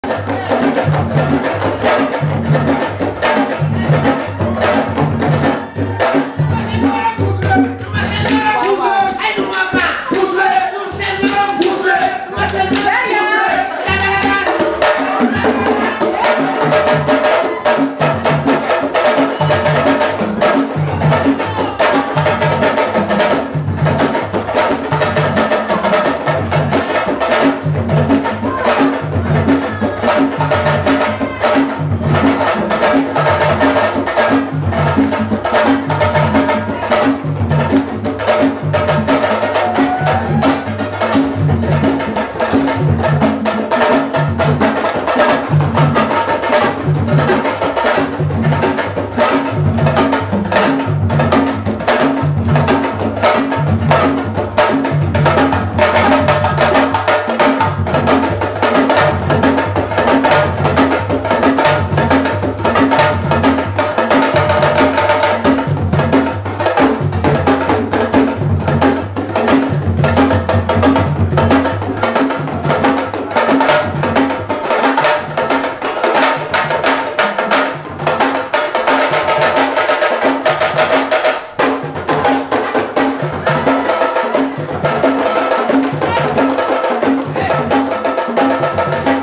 Aziz Faye leads steps in dance class at Camp Fareta 2013
Aziz Faye, master dancer from Senegal, West Africa, leads the men across the floor in a sabar dance class at Camp Fareta 2103 in California. The drummers are Cheikh M'Baye, Ibou Ngom, and Omar Mboup.